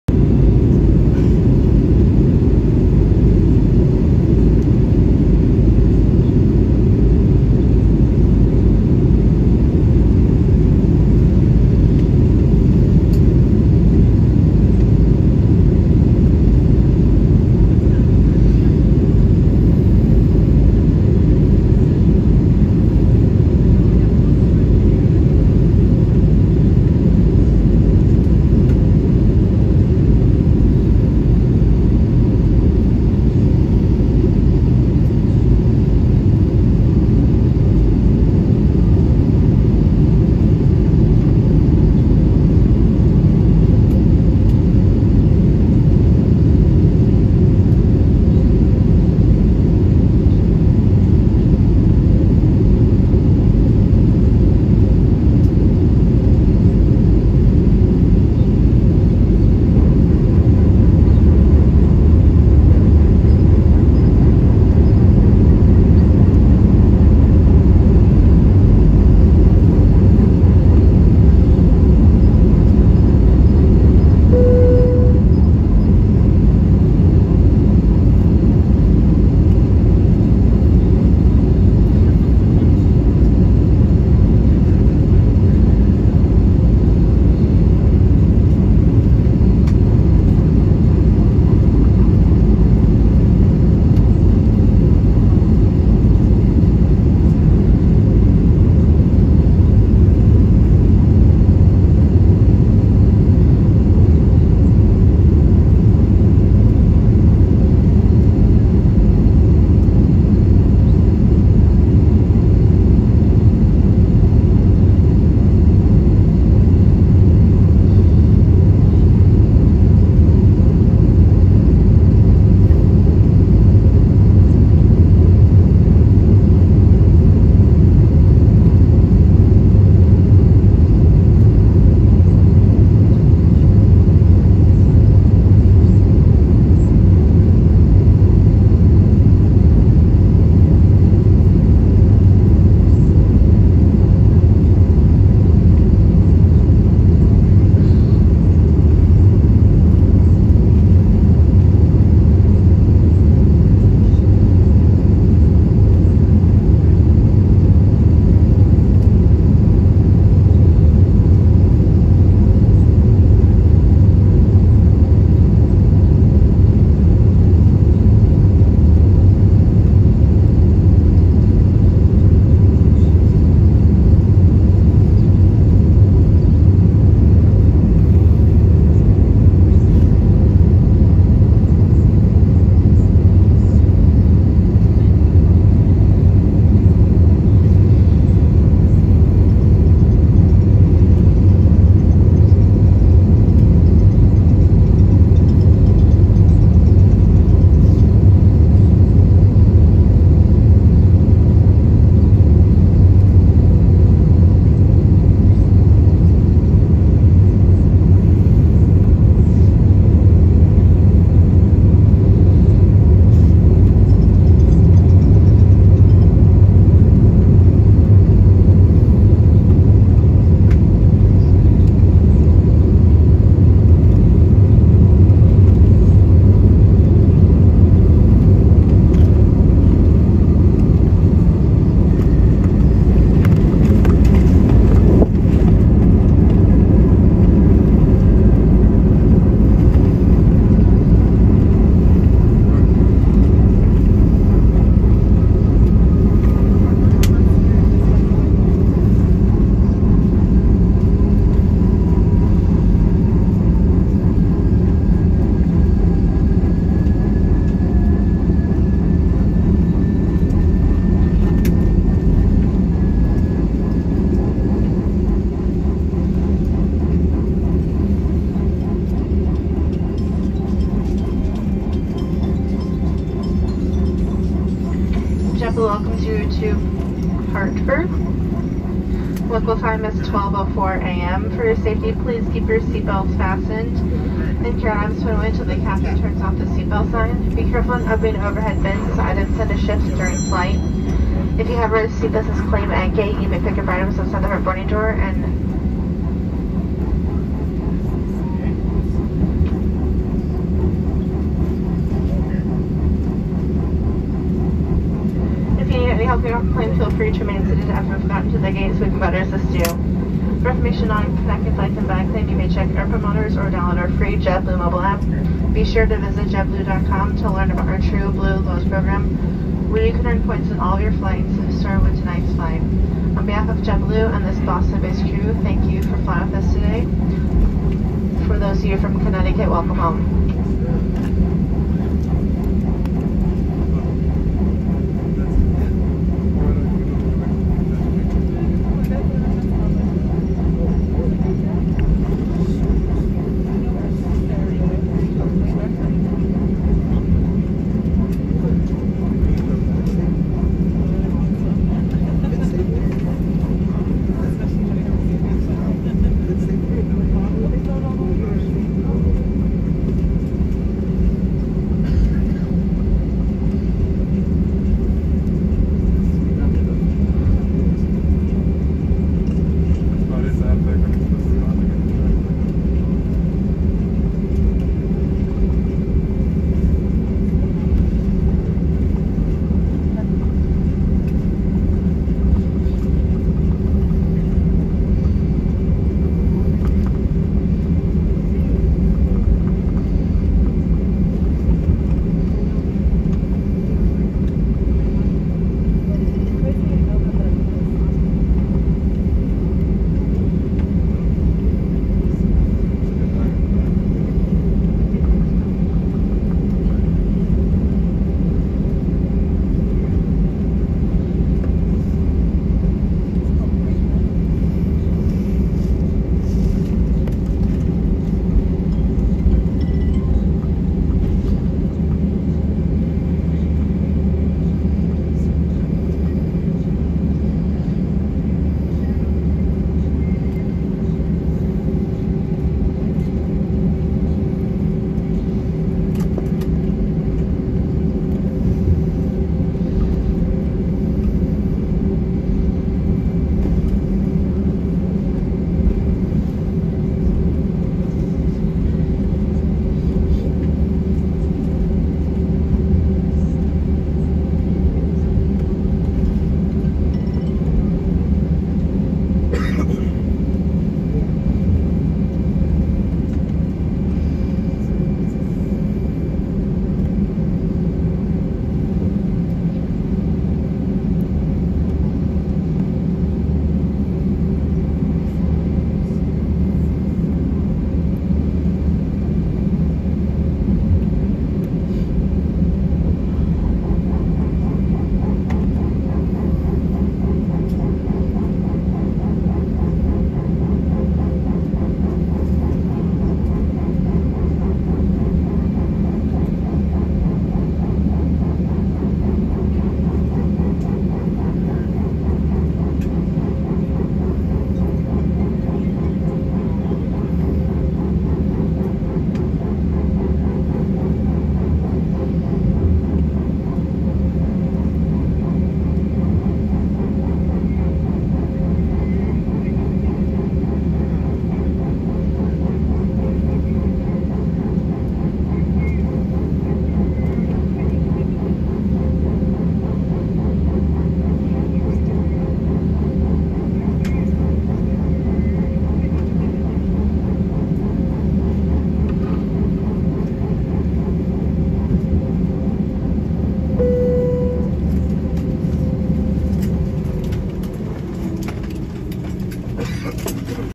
JetBlue Airways Airbus A320-200 Landing in Hartford/Springfield
Date: 7/16/21 (technically)
Flight: B6460
Route: FLL-BDL
Plane: A320-232
Registration: N571JB
Aircraft age at flight: 17.8 years old
Seat: 23F
Runway used: 24

New videos are posted every Friday at noon EST.